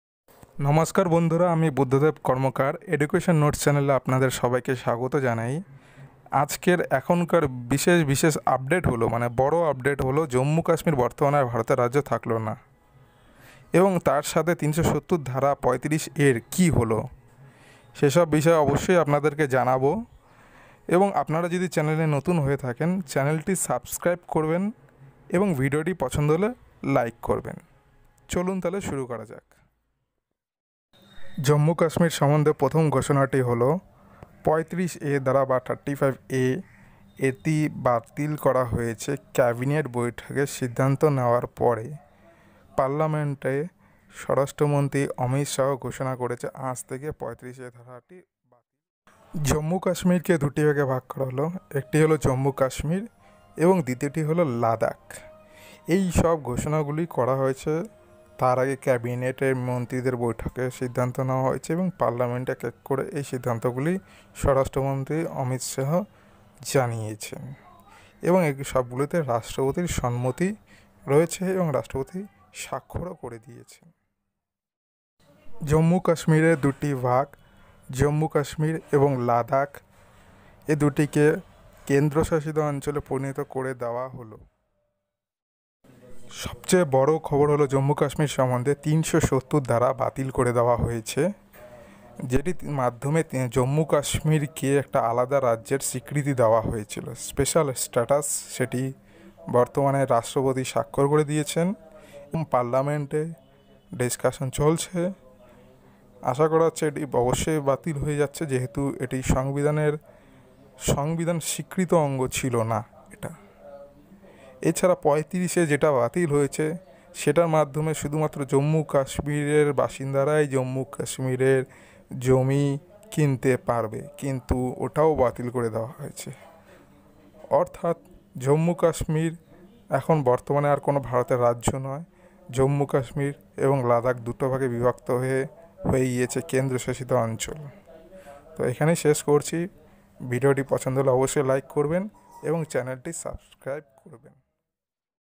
Jammu Kashmir Big Breaking | Art 370 & 35A Abolish | জন্মু কাশ্মীরের বড় পরিবর্তনএক নজরে জেনে নিন
জন্মু কাশ্মীর আর ভারতের রাজ্য নয়, বড় পরিবর্তন এলো
Article 370 , Article 35A